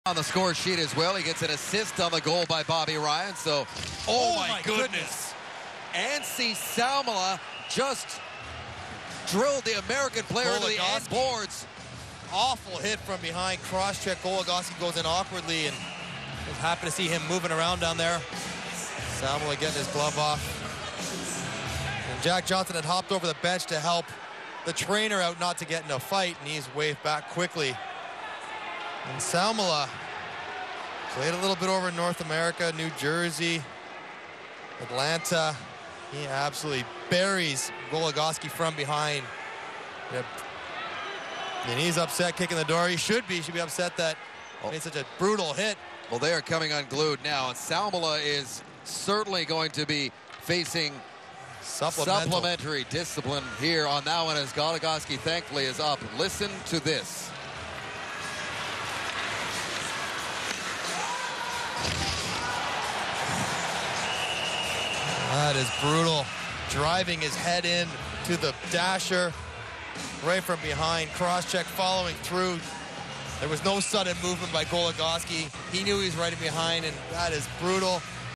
Alex Goligoski boarded by Anssi Salmela at World Championships
Follow the CBSSports.com Eye on Hockey blog on Twitter @EyeOnHockey